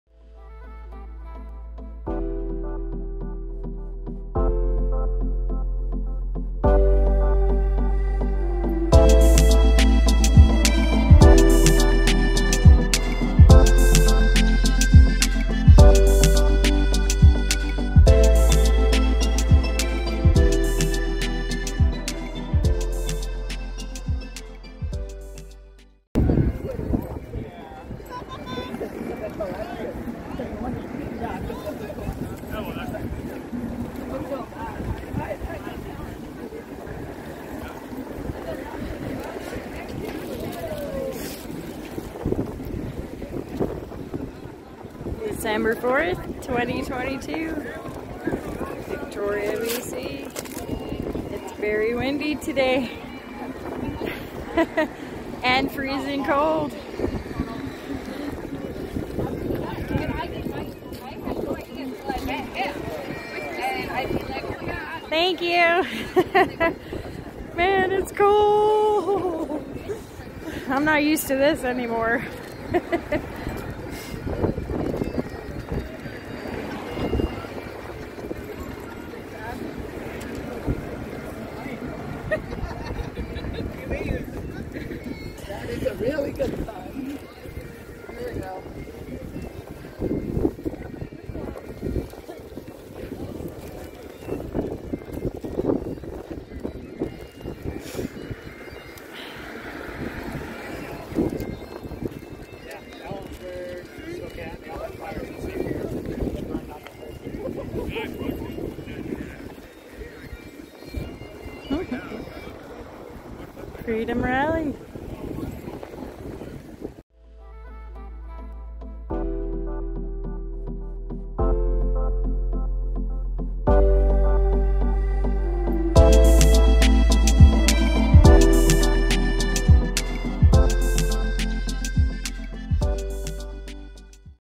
Freedom Rally Victoria Bc (December 3, 2022) Sign Walk
Freedom Rally
Victoria Bc
(December 3, 2022)
Sign Walk

💎NO FARMERS NO FOOD 💎
Rally to support the Canadian and Dutch Farmers, Truckers, and Freedom For All
Stay Blessed and Keep Standing Strong